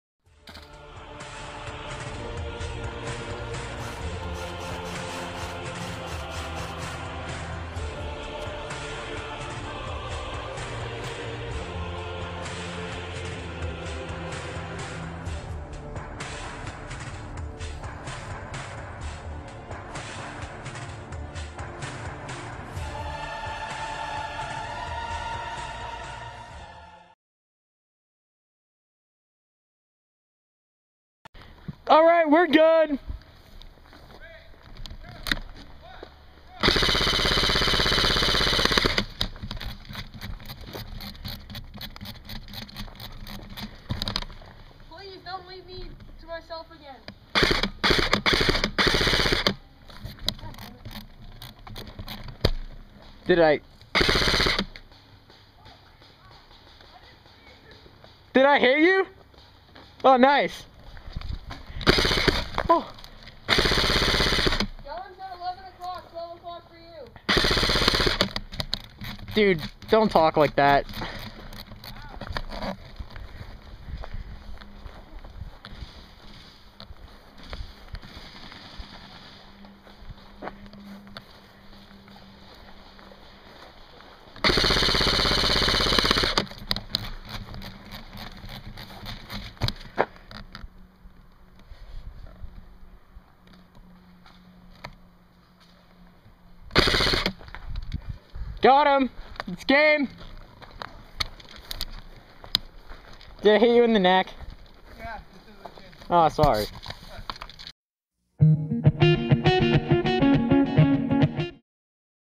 2v3 CQB Airsoft At Midway Paintball Duncan BC ''Tire Field''
Quick game in the Tire Field at Midway Paintball from the June 26th 2011